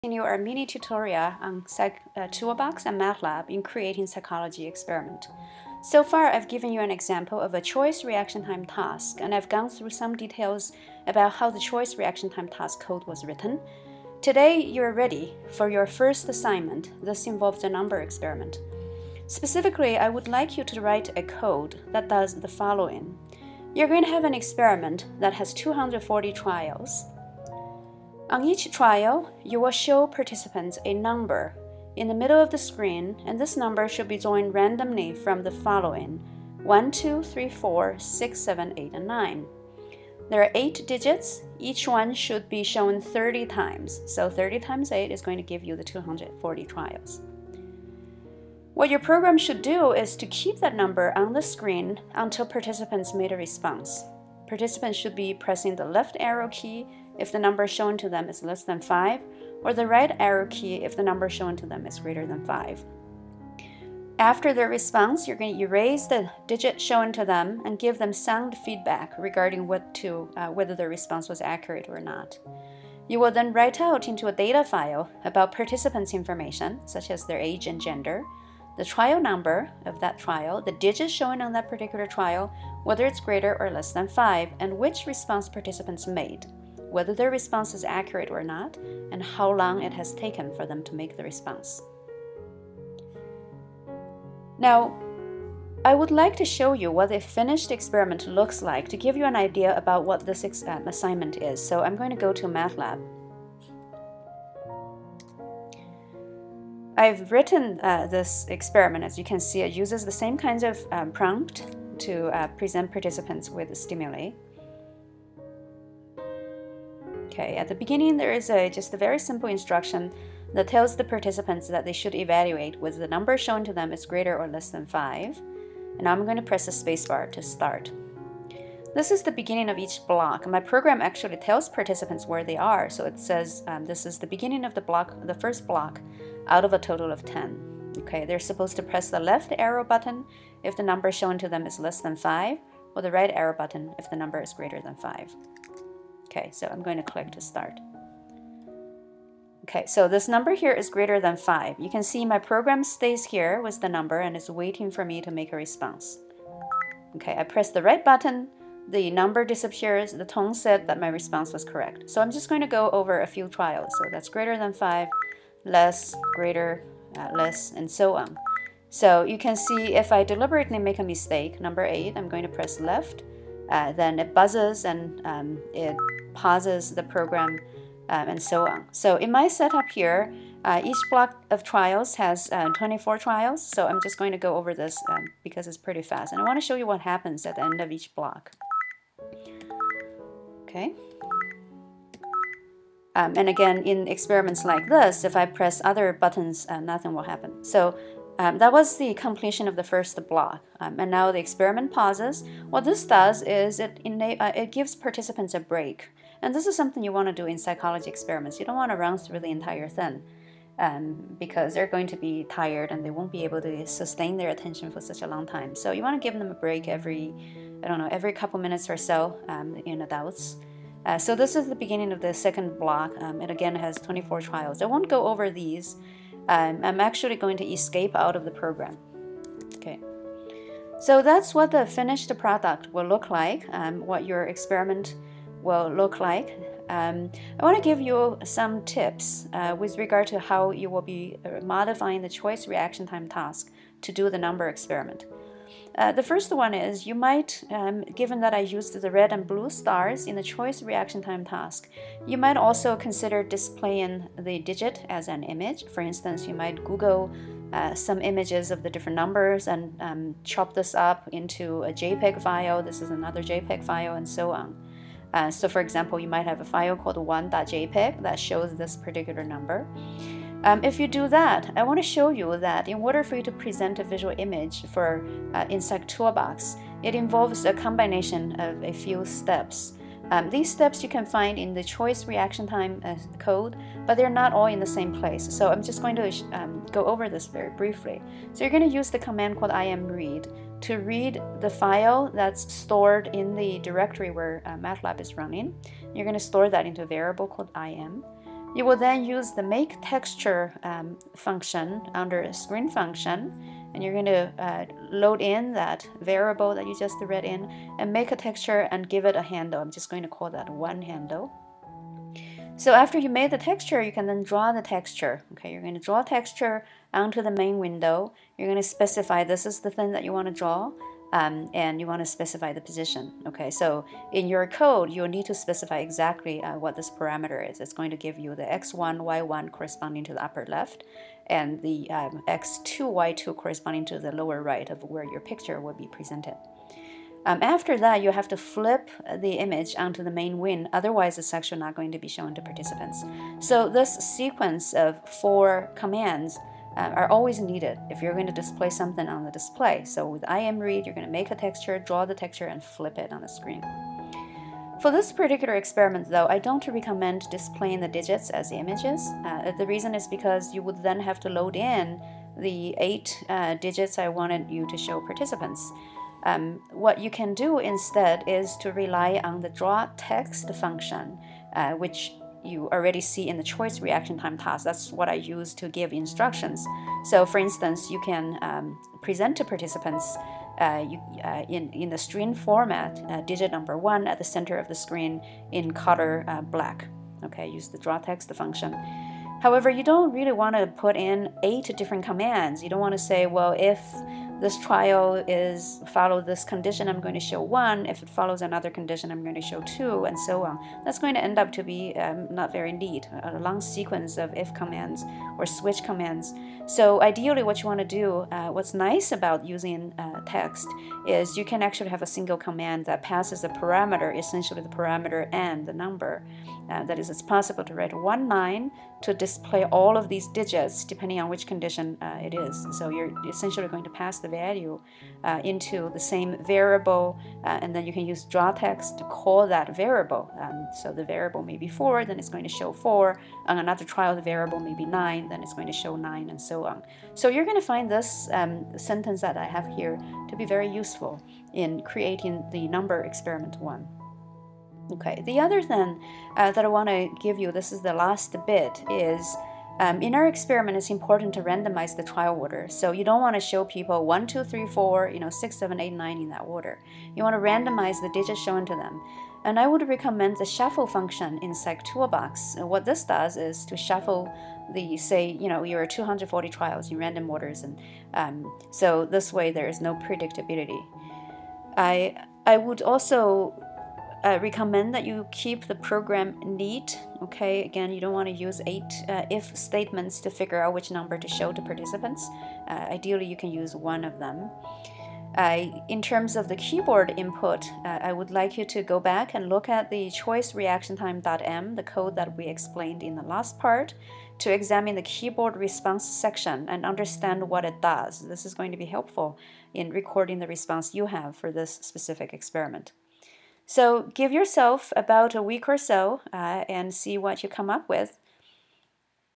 MATLAB & Psychtoolbox Tutorial Part4: Assignment #1 - Number Exp1
Part 4: Want to try to code a cognitive experiment? Here’s your first assignment and a demo of what the assignment looks like. You will also learn some tips about what to do for this assignment.